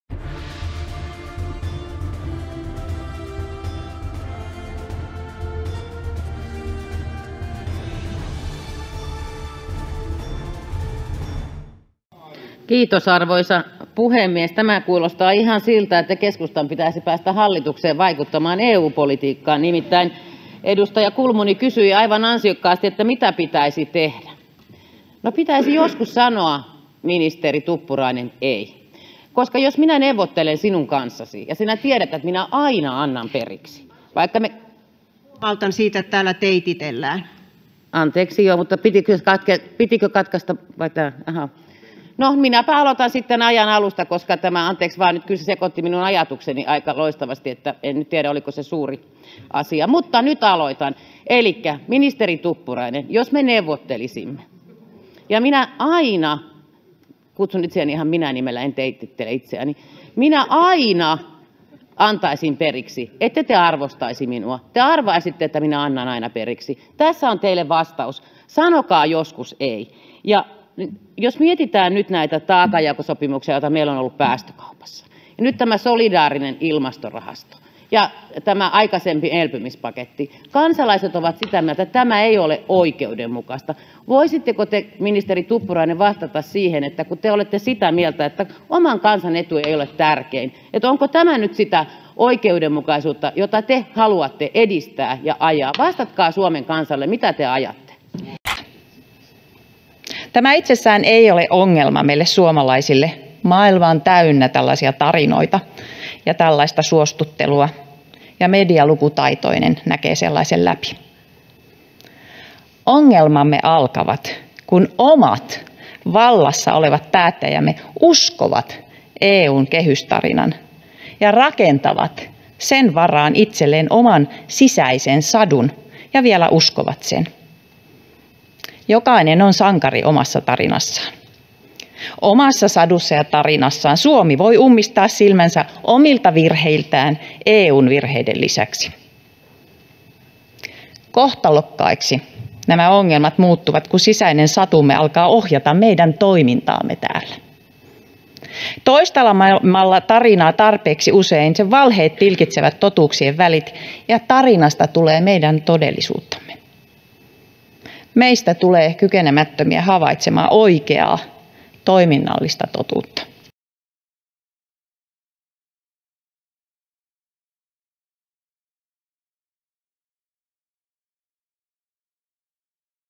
Perussuomalaisten Leena Meri ja Lulu Ranne "kiusaa" hallituksen edustajia.
Lulu:
Tämä itsessään ei ole ongelma meille suomalaisille. Maailma on täynnä tällaisia tarinoita ja tällaista suostuttelua, ja medialukutaitoinen näkee sellaisen läpi. Ongelmamme alkavat, kun omat vallassa olevat päättäjämme uskovat EU:n kehystarinan ja rakentavat sen varaan itselleen oman sisäisen sadun ja vielä uskovat sen. Jokainen on sankari omassa tarinassaan. Omassa sadussaan ja tarinassaan Suomi voi ummistaa silmänsä omilta virheiltään EU:n virheiden lisäksi. Kohtalokkaiksi nämä ongelmat muuttuvat, kun sisäinen satumme alkaa ohjata meidän toimintaamme täällä. Toistelemalla tarinaa tarpeeksi usein sen valheet tilkitsevät totuuksien välit ja tarinasta tulee meidän todellisuuttamme. Meistä tulee kykenemättömiä havaitsemaan oikeaa toiminnallista totuutta.

Ja kun me emme näe sitä todellisuutta, se yllättää meidät kerta toisensa jälkeen, aivan kuten Kreikan kirjanpito yllätti, kuten Afganistan yllätti, kuten maahanmuuttajataustainen rikollisuus, jengiytyminen ja terrorismi ovat yllättäneet, kuten nyt energian hinta yllätti ja kuten Italia tulee yllättämään. Oikea todellisuus ei piittaa meidän saduistamme ja tarinoistamme. Siksi teemme lisää virheitä ja peitämme ne kasvattamalla ja laajentamalla valheellista tarinaa, kunnes se ei enää etäisestikään muistuta oikeaa todellisuutta.

Leena:
Kiitos, arvoisa puhemies! Kysymyshän on siitä, mikä on sen kunnianhimon hinta. Te totesitte, pääministeri, että Suomi ei tyydy eikä lähde mukaan elpymispaketteihin, mutta esimerkiksi Kauppalehden pääkirjoituksessa 25.10. on todettu, että tässä on elpymispaketin elementtejä. Silti teidän kantanne on se, että te ette sano ehdottomasti ”ei” vaan selvitätte jotain. Mitä te nyt selvitätte? Ettekö te voi yksinkertaisesti ilmoittaa, että Suomen viiden miljoonan kansa ei aio osallistua Euroopan 450 miljoonan ihmisen sosiaalitukirahastoihin tukemaan heidän elämäänsä, vaan me haluamme, että varat käytetään suomalaisten tukemiseen, heidän elämässään sähkölaskujen ja liikenteen tukemiseen? Eli kysyn: Miksi te ette sano nyt ”ei”, kun tässä on elpymispaketin elementtejä? Mitä te nyt selvitätte?